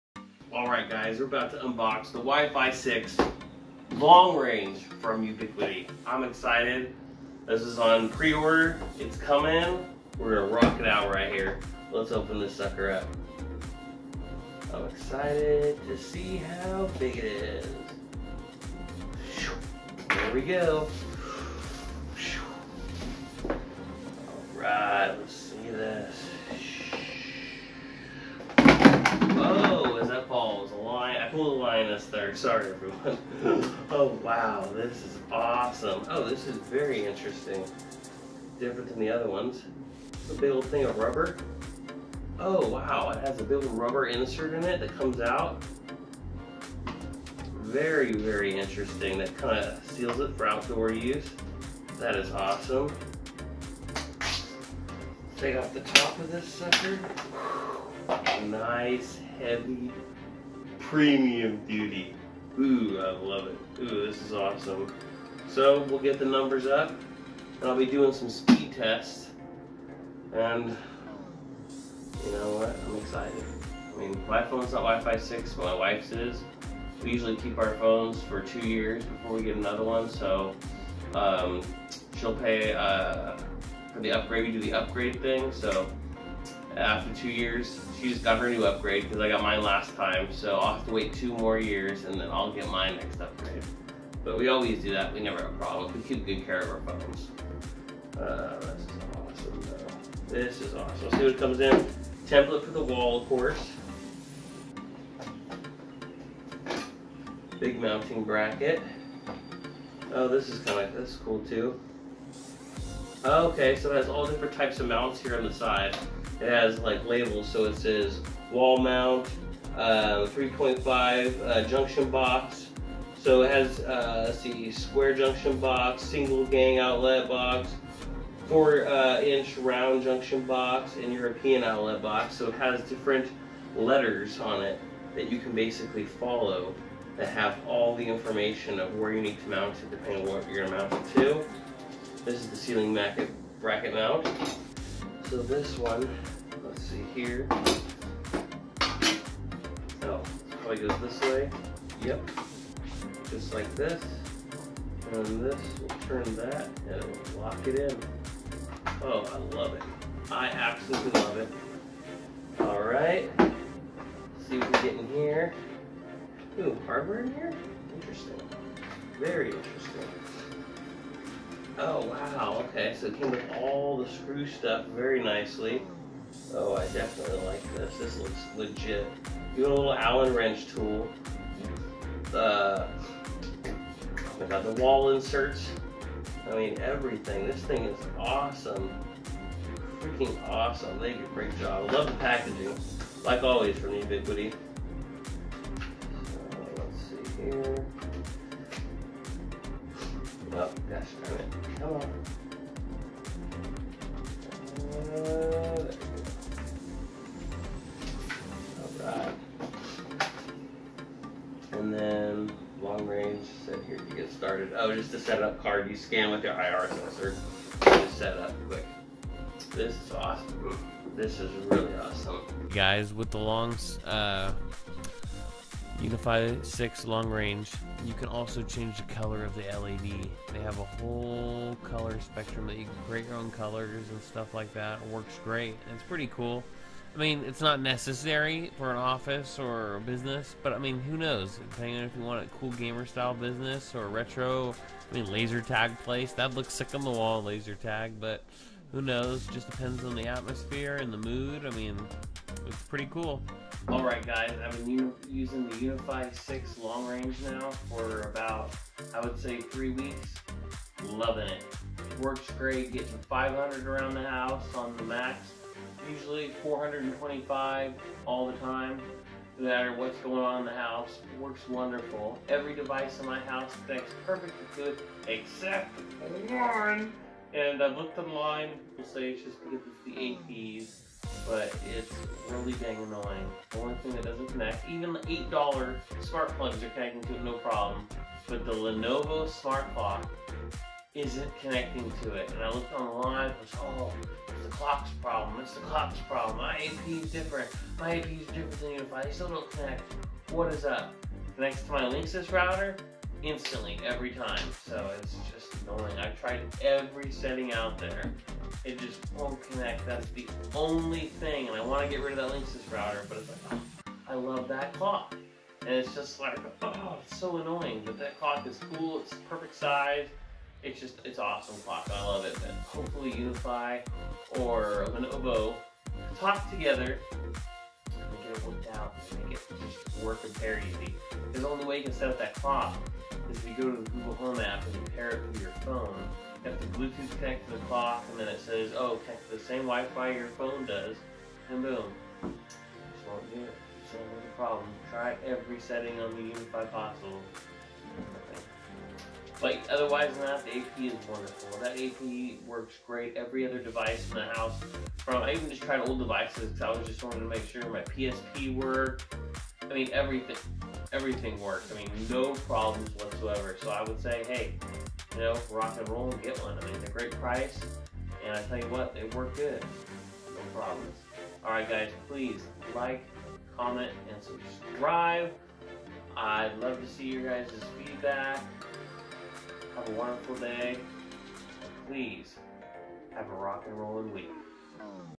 UniFi 6 Long Range AP
UniFi 6 Long-Range AP review

Just wanted to let everyone know the Lenovo smart clock works now they had a new firmware update for the UDM pro and after I installed that I did try creating a VLAN and making an IOT one and boom after I said all my other IoT devices up on my other Google stuff The clock worked I don't understand but hey it works this is on the fourth day with no problems